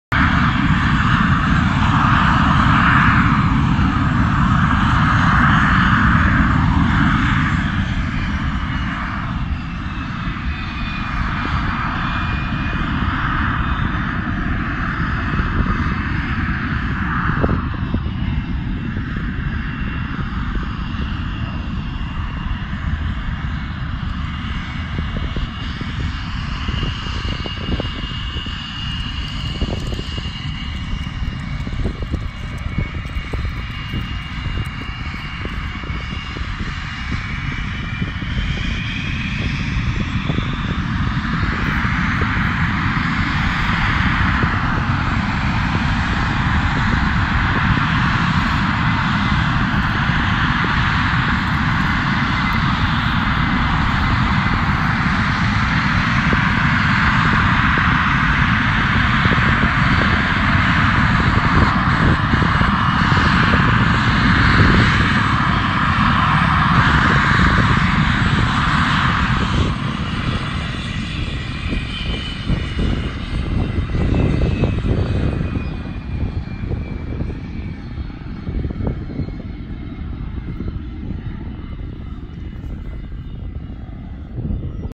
Lightning422 taxiing down Stennis International active runway
This is the first time since 1987, that Lightning422 has been taxied down an active runway. It is a major milestone in her restoration  programme back to flight.